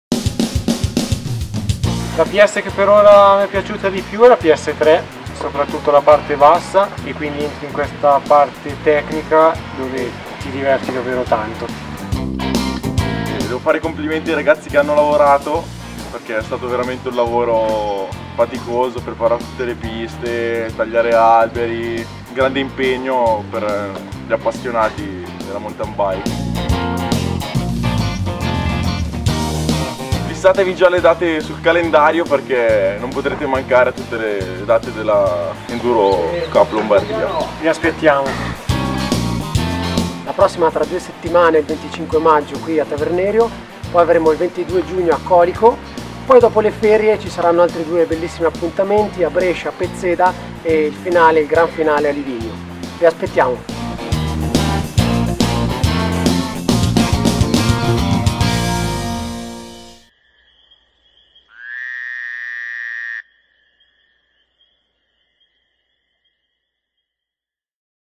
Enduro Cup Lombardia 2014
Enduro Cup Lombardia Stagione 2014
Riprese e montaggio di Riccardo Ugo

L'Enduro Cup Lombardia riparte dallo stesso punto dove, nel 2013, tutto aveva avuto inizio. Nata come scommessa in un'area che rappresenta una gran fetta del mountain biking italiano, l'Enduro Cup Lombardia si ripresenta per il 2014 in grande stile, affiancando nuovi appuntamenti alla piacevole sorpresa di Tavernerio.
Il trofeo 2014 si articola su 4 prove, con apertura a Tavernerio (CO), salendo poi a Colico (LC), quindi Pezzeda (BS) e infine Livigno (SO).
Un crescendo di emozioni che porterà concorrenti e pubblico dalle Prealpi Lombarde alle alte montagne della Valtellina, con l'assegnazione della maglia di Campione Regionale in prova unica durante la prova di Livigno, e l'assegnazione del titolo di Campione del trofeo Enduro Cup Lombardia.
Quattro location per altrettante gare uniche: dal flow di Tavernerio ai sentieri tecnici di Colico, le lunghe discese di Pezzeda e i percorsi tanto amati di Livigno.
Ecco le date:
- 25 Maggio TAVERNERIO
- 22 Giugno COLICO
- 31 Agosto PEZZEDA
- 13-14 Settembre LIVIGNO

Riders del video: Lorenzo Sorrentino Campione DH 2013, Michele Gibelli, Federico Valenzisi Campione ECL 2013, Caludio Riotti di e Luca Trevisan di